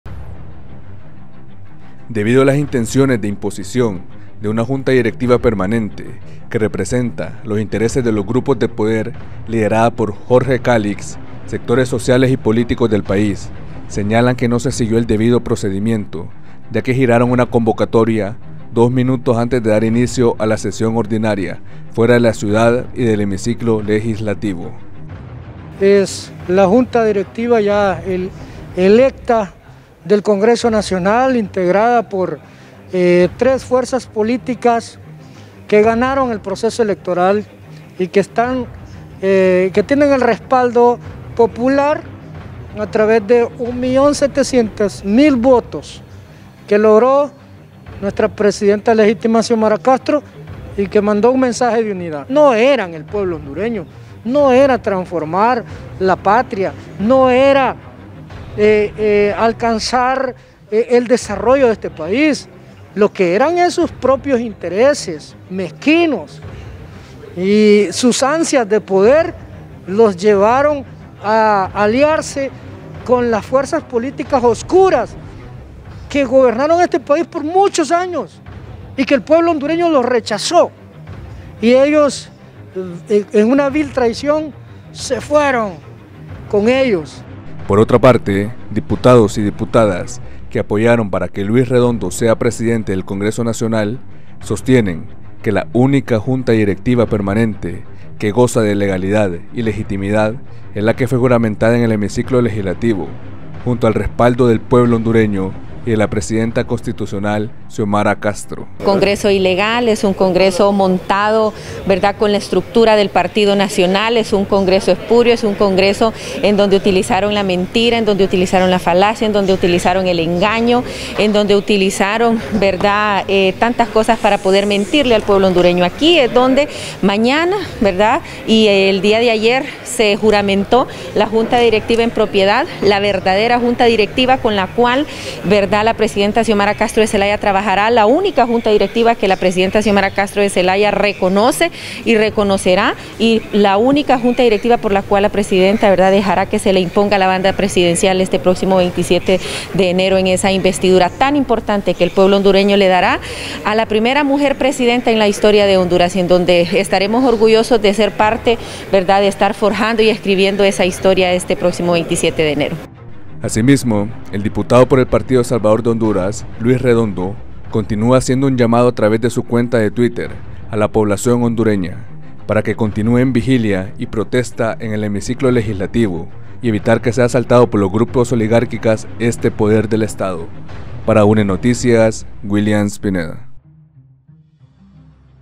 UneNoticias | Política 🔴👇
Ante la intención de imposición de la junta directiva encabezada por Jorge Cálix, sectores políticos del país señalan que es ilegal ya que realizaron la sesión fuera del hemiciclo y es rechazada por el pueblo hondureño.